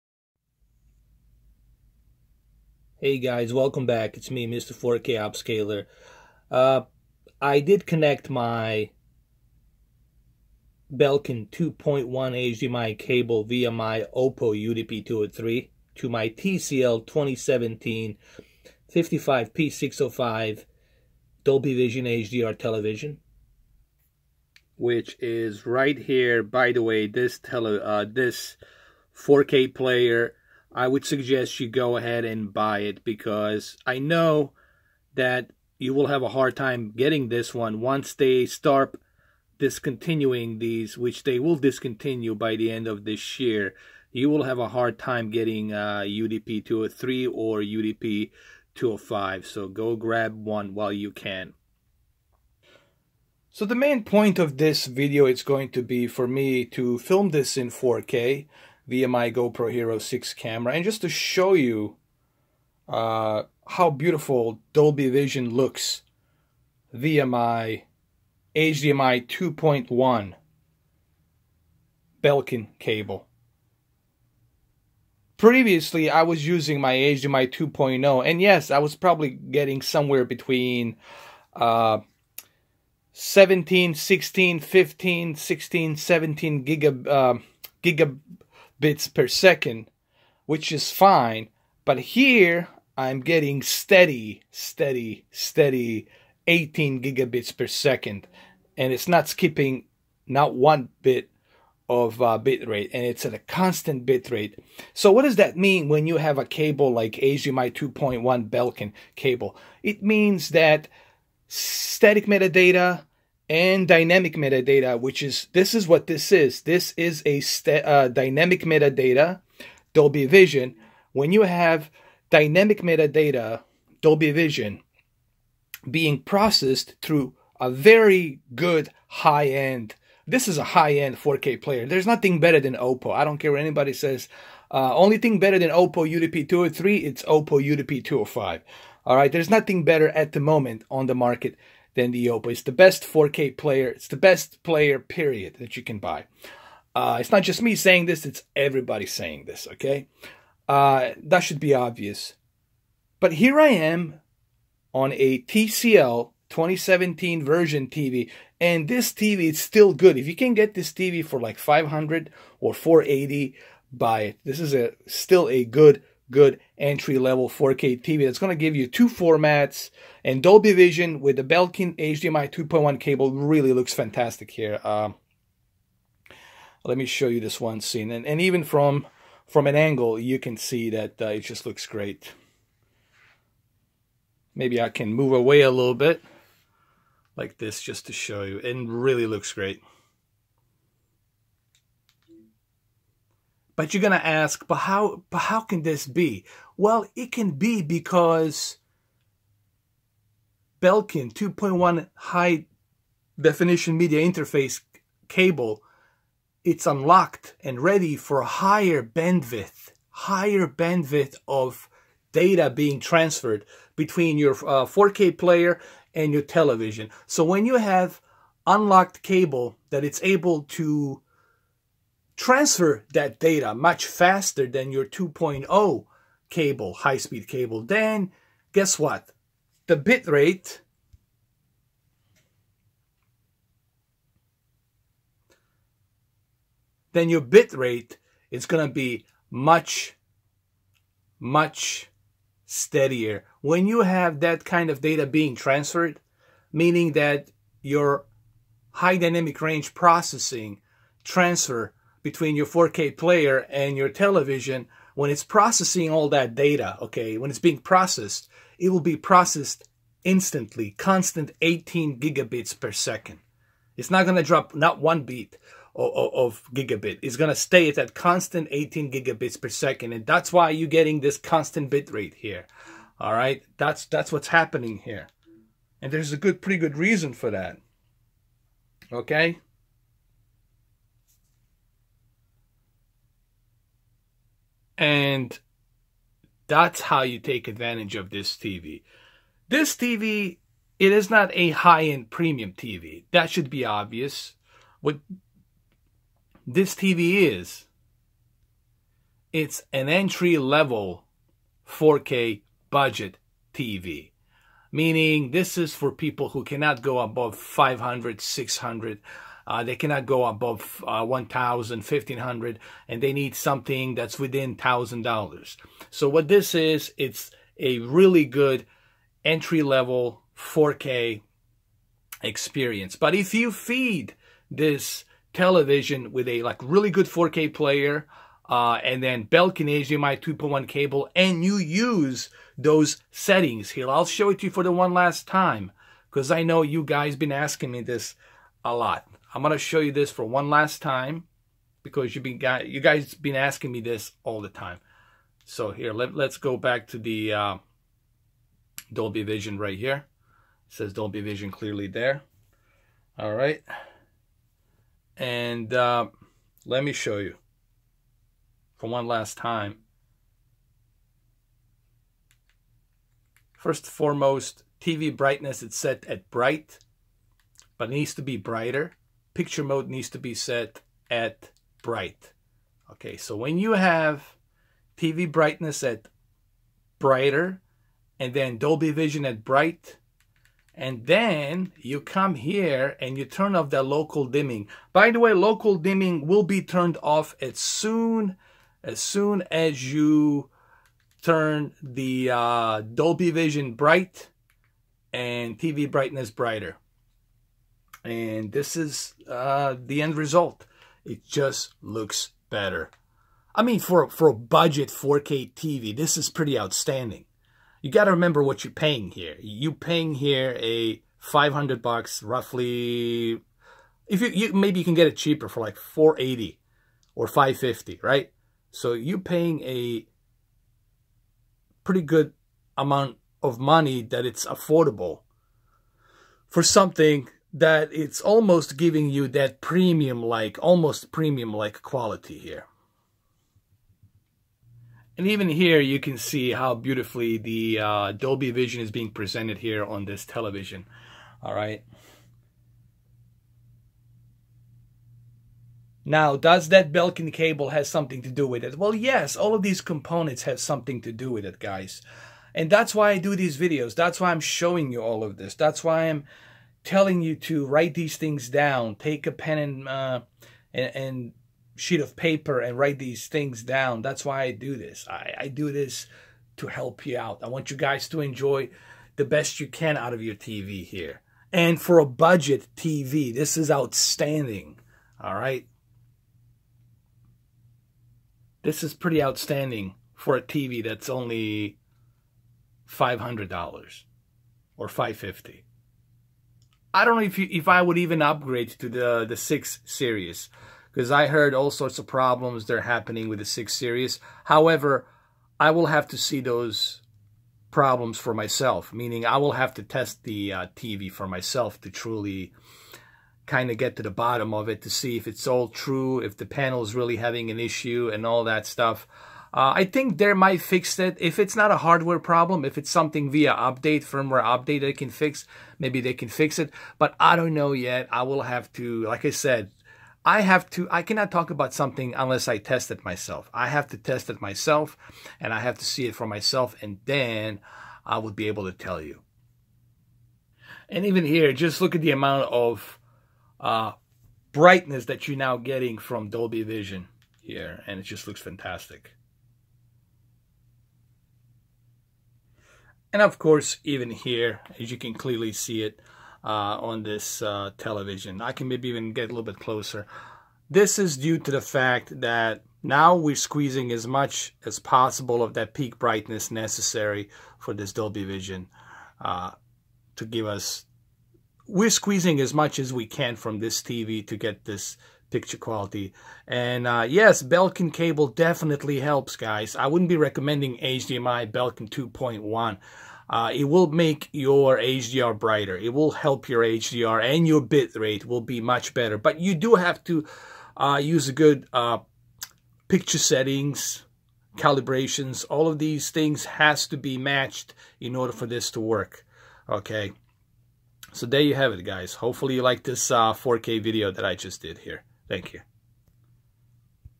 Dolby Vision & Belkin 2.1 via Oppo UDP 203 & TCL p605 4K TV
Does Belkin 2.1 hdmi cable helps Dolby Vision format be more constant in rendering that format , via it's more instant & steady bandwidth of 18gbps data .Well that's what will talk about on this video,  & yes this video is recorded in Native 4K .